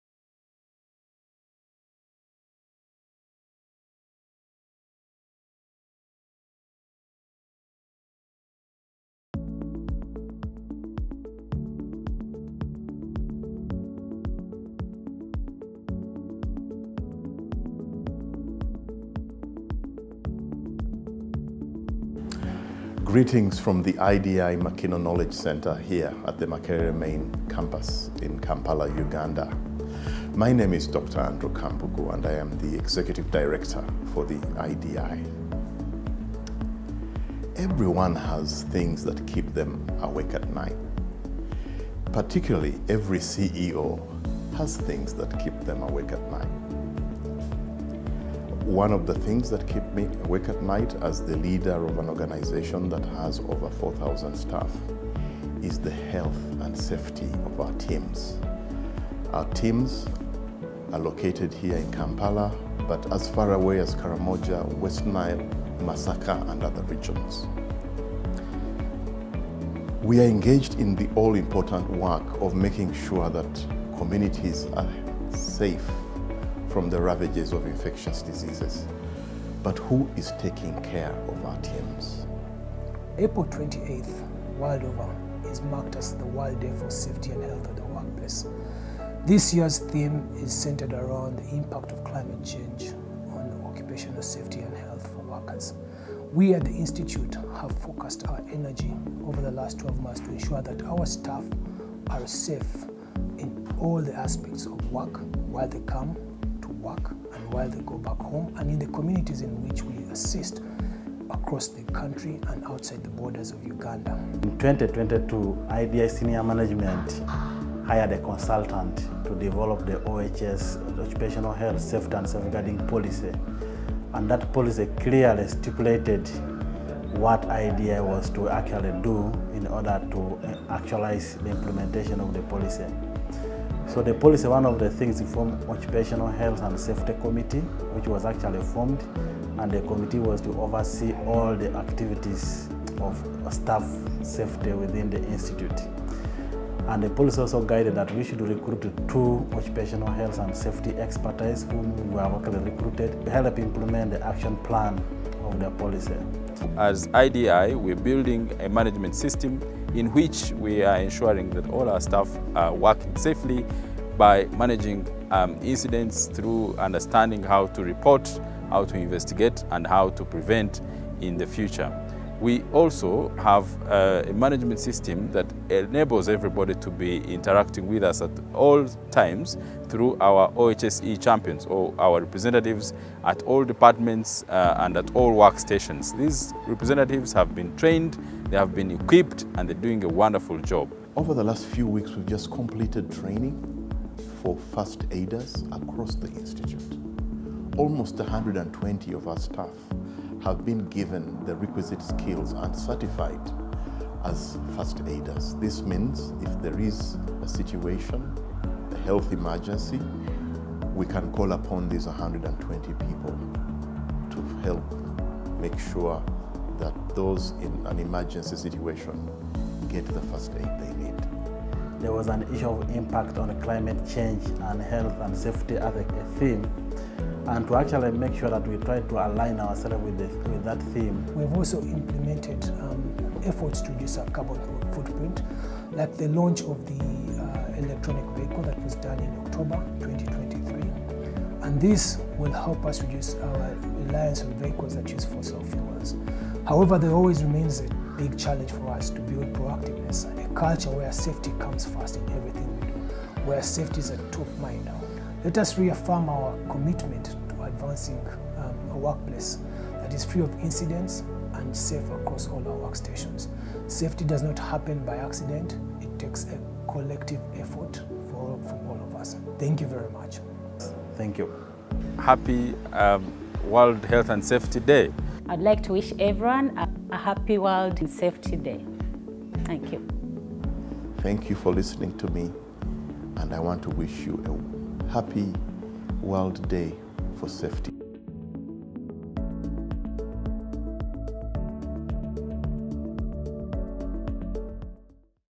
Infectious Diseases Institute World Health & Safety Day 2024 Commemoration
To strengthen health systems in Africa, with a strong emphasis on infectious diseases, through research and capacity development.